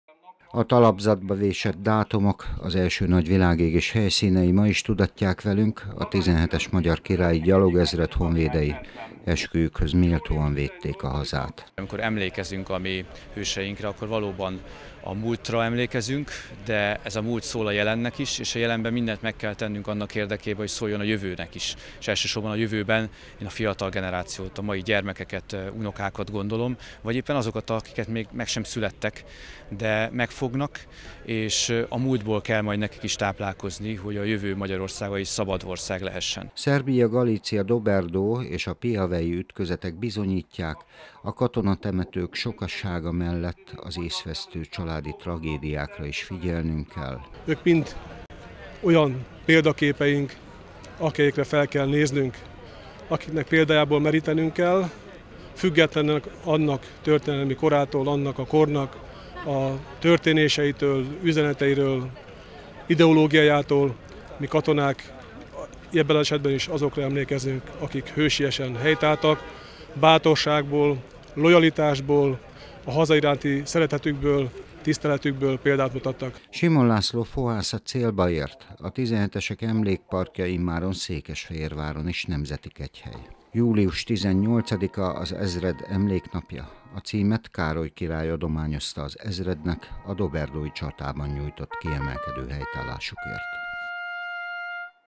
Koszorú a 17-eseknek
Július 18-a, a Magyar Királyi 17-es gyalogezred emléknapja. A kitüntető címet az első világháborúban hősiesen helytálló magyar honvédeknek még Károly király adományozta. Kései utódaik nem felejtették el a hősök emlékét, ma délután a városi és a megyei önkormányzat vezetői, az Összhaderőnemi Parancsnokság és a civil szervezetek helyezték el a megemlékezés koszorúit a Zichy ligetben, a 17-esek emlékparkjában.